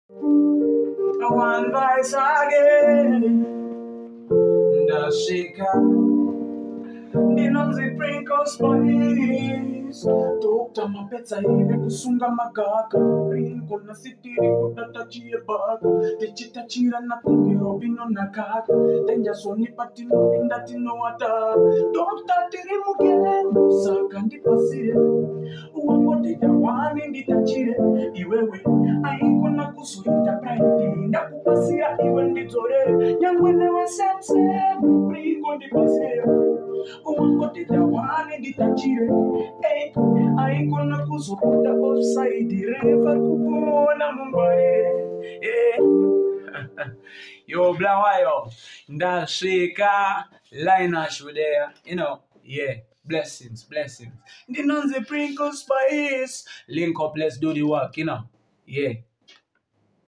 Princo Spice ( Musamurai ) in the studio with Laynash_skyz ( at Impala RecordsZw ( Bulawayo )
Studio Sessions with Princo Spice